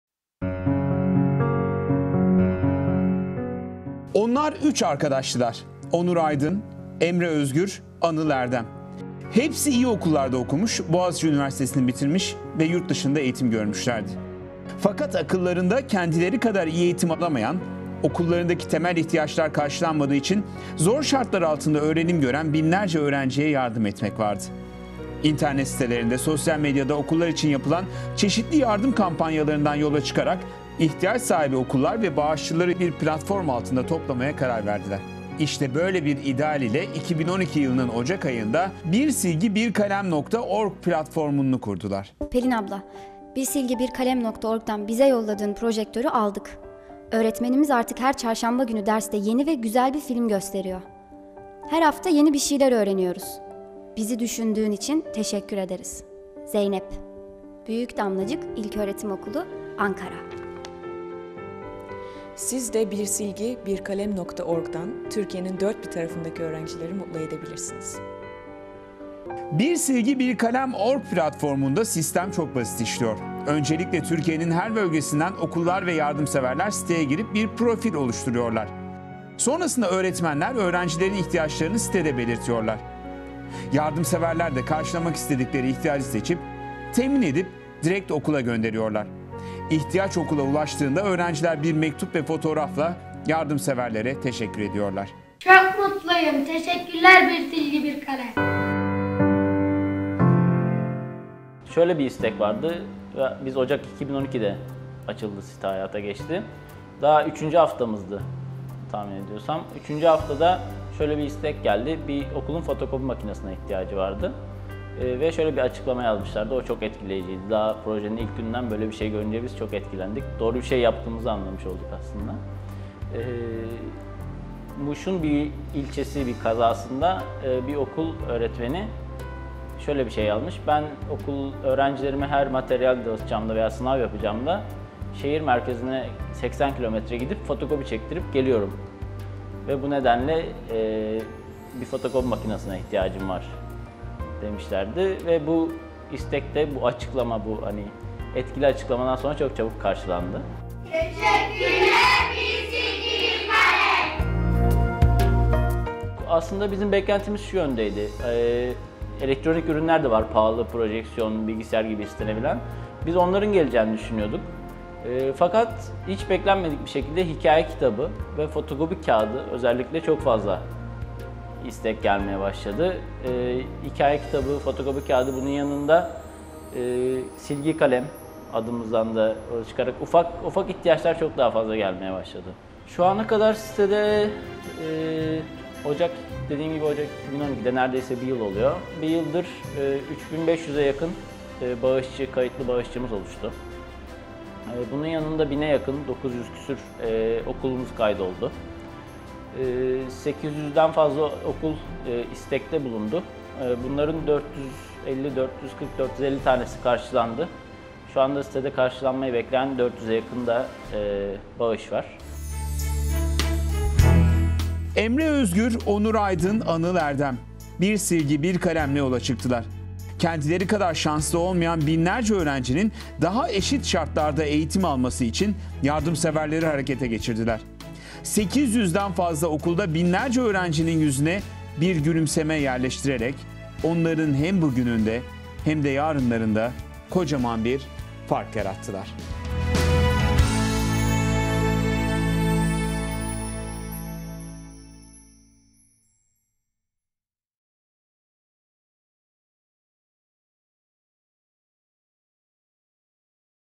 Fark Yaratanlar | Bir Silgi Bir Kalem Projesi | 4. Sezon
"Bir Silgi Bir Kalem Projesi" Bağışçılarla İhtiyaç Sahibi Okulları Buluşturuyor

Sabancı Vakfı, “Fark Yaratanlar” programının yeni konuğu, ihtiyacı olan okullarla, onlara destek olmak isteyen bağışçıları, sanal ortamda buluşturan Bir Silgi Bir Kalem Projesi…

Bir Silgi Bir Kalem

Sabancı Vakfı

Fark Yaratanlar